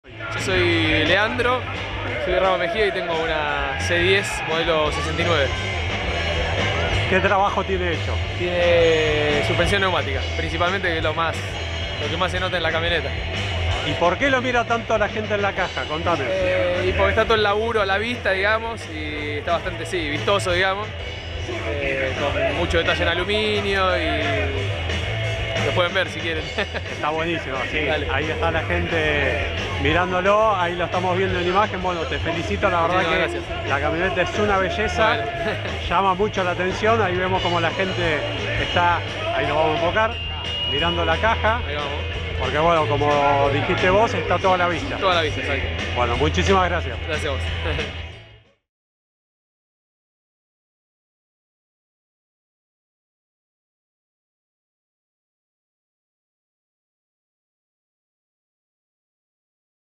Camioneta CHERVOLET C10 Modelo 1969 con suspensión neumática y todo a la vista
Lean estuvo en el encuentro que realizó la gente de La Cromada Hot Rod y nos presentó su hermosa camioneta CHEVROLET C10 DEL AÑO 1969 con suspensión neumática y un trabajo impresionante, todo a la vista!
Un proyecto impresionante que dejó a toda la gente con la boca abierta!